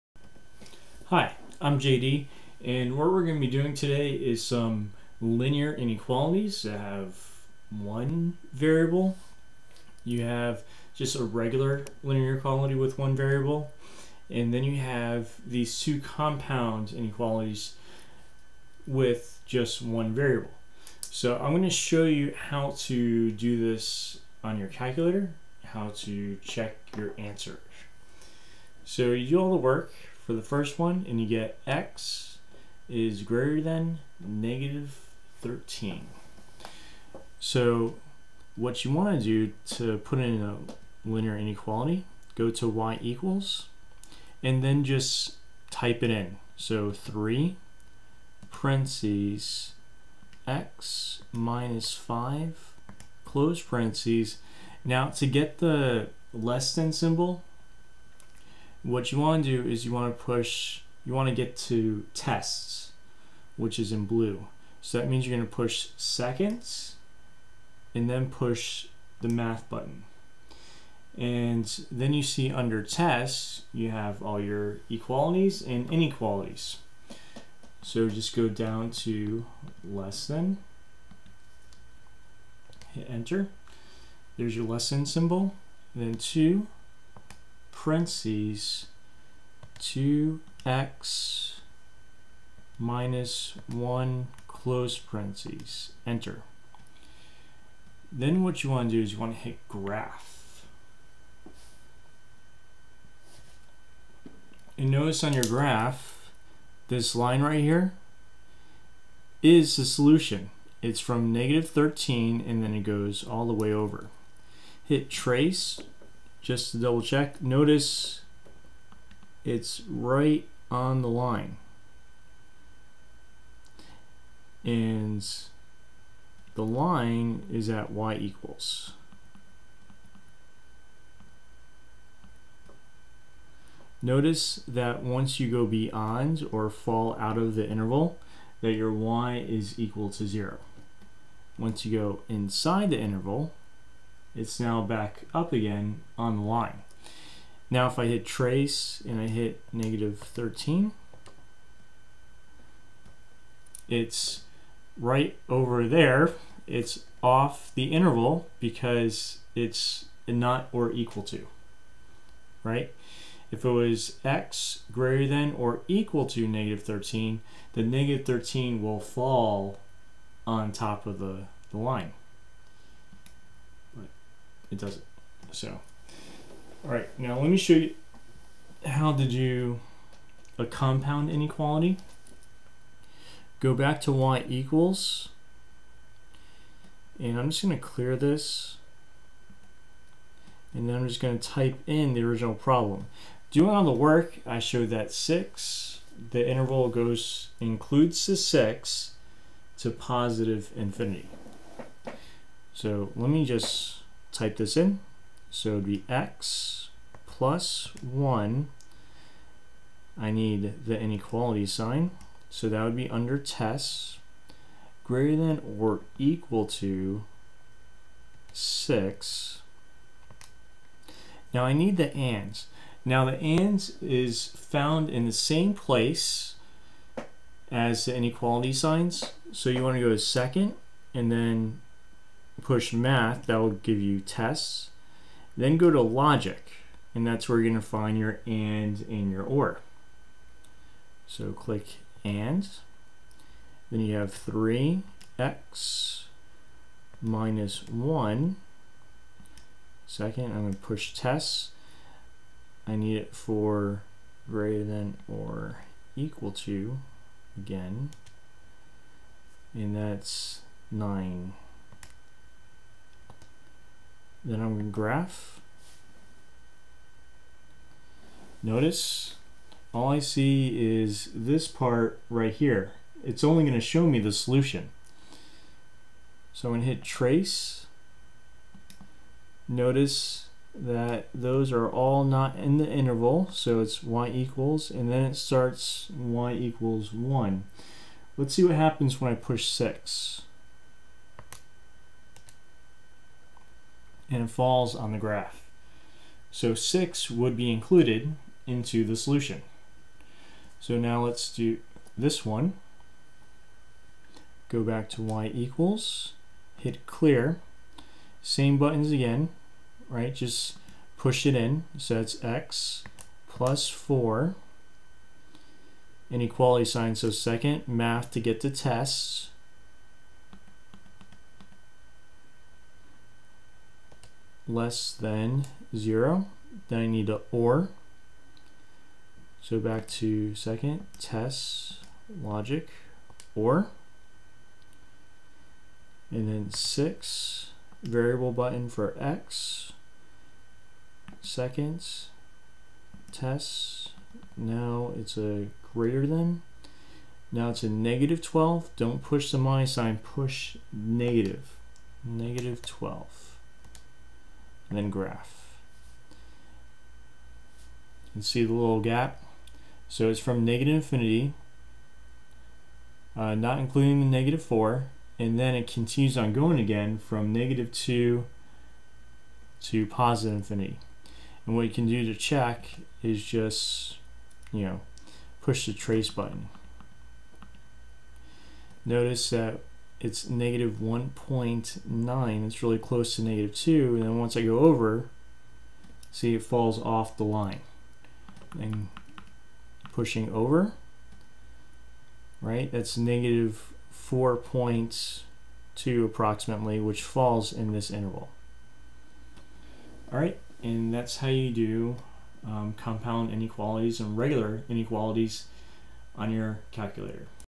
algebra - linear inequalities in one variable - TI-84 - examples
Using a TI-84, I show to put linear inequalities with one variable into the calculator to verify your solutions. One example of a linear inequalities and two examples of compound linear inequalities.